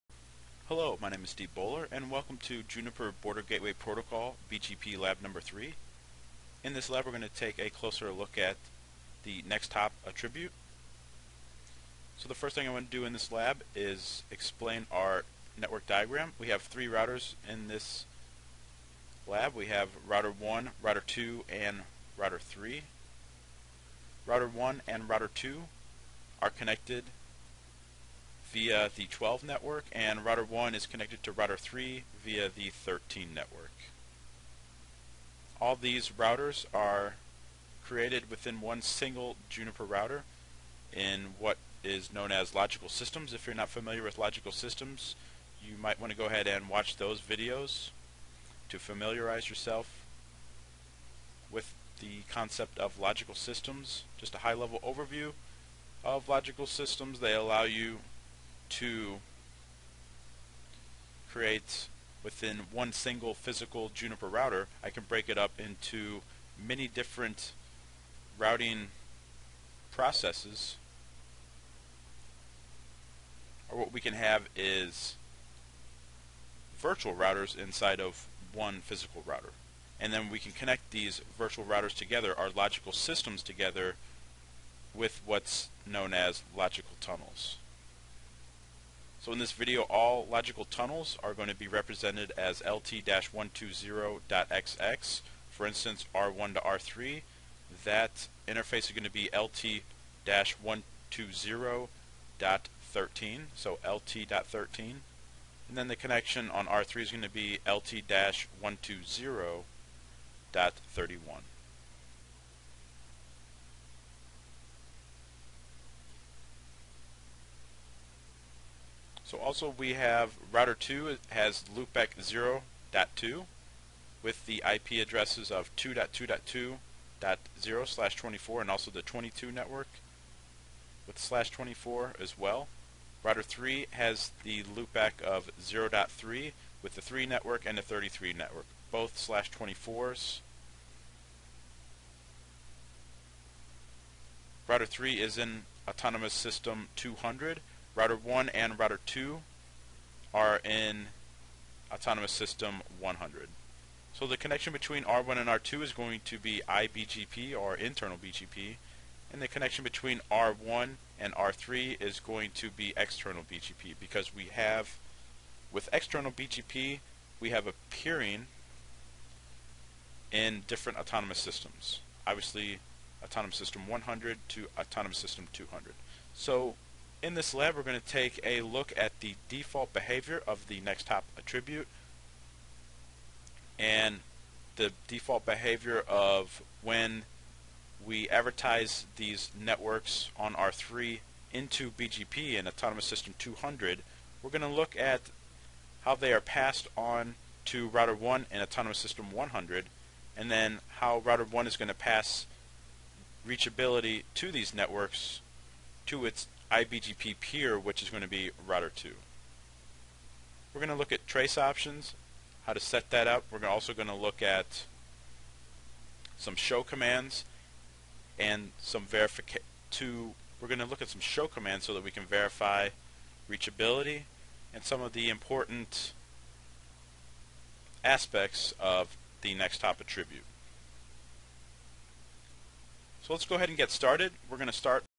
Juniper BGP Lab 3 Next-Hop Attribute
In this video I configure and explain the following:

-The basic principles of the Next Hop BGP attribute
-How to setup Next Hop Self
-Why you would use Next Hop Self
-Use show commands to verify proper Next Hop Self operation